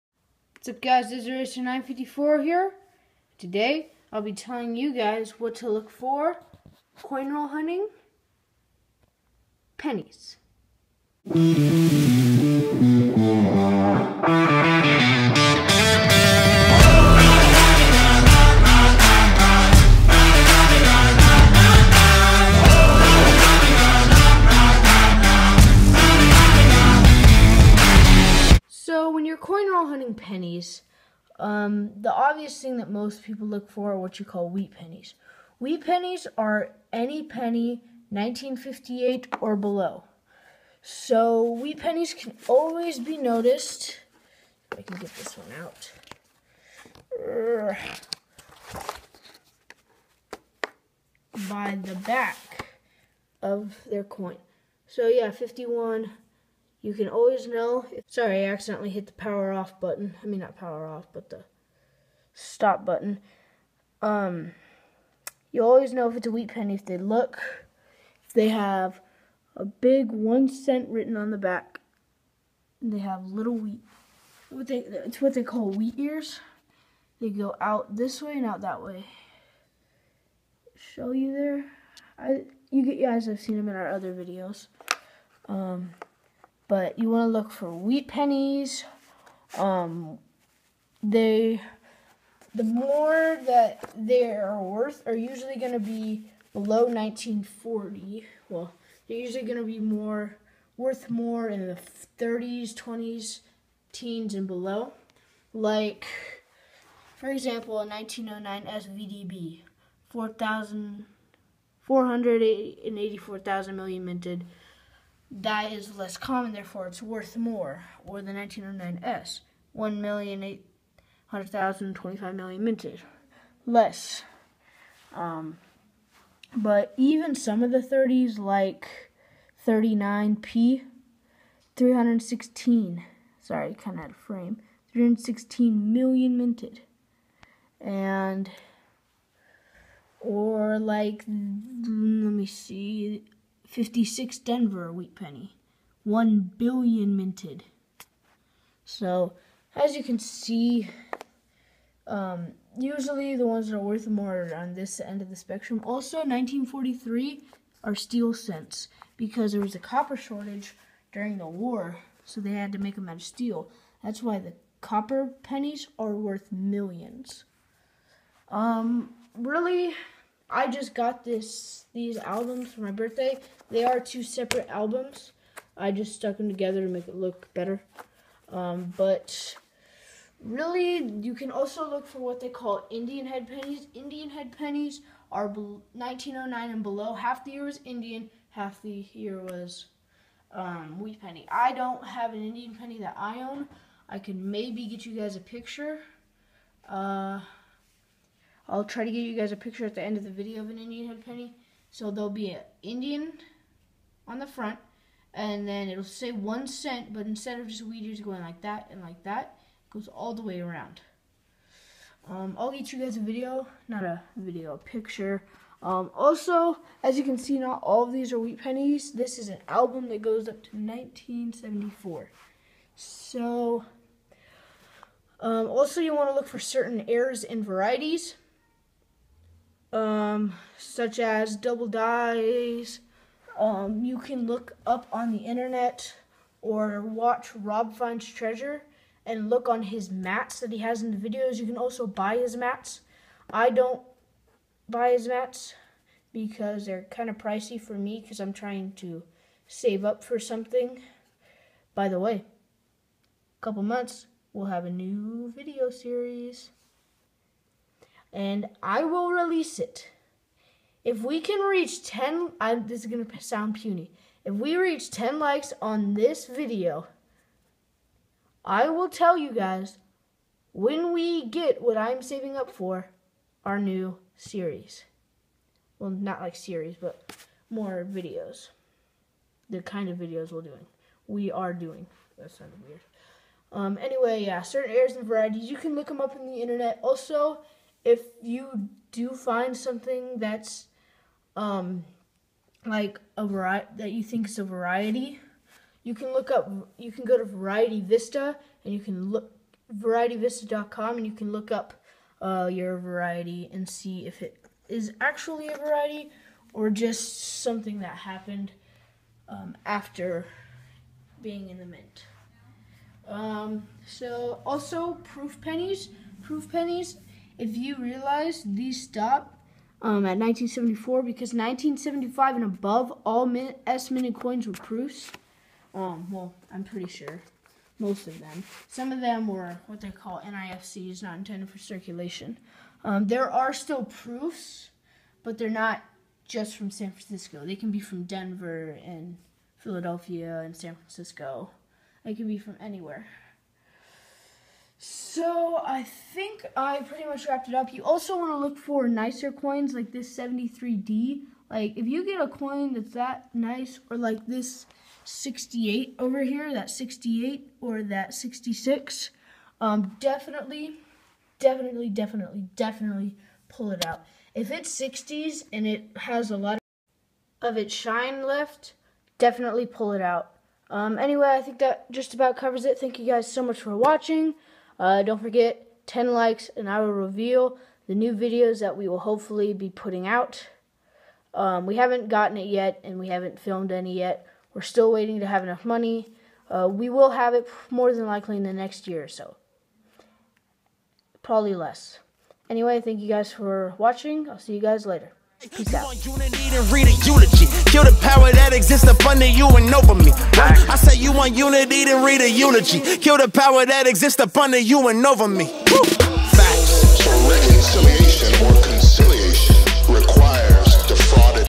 What To Look For Coin Roll Hunting Pennies
Today I will be sharing with you guys what you should be looking for a coin roll hunting pennies. It's been a struggle for me to find videos to film. By the way don't forget if we get 10 likes on the video I Will reveal to you guys the new series of videos we will be coming out with.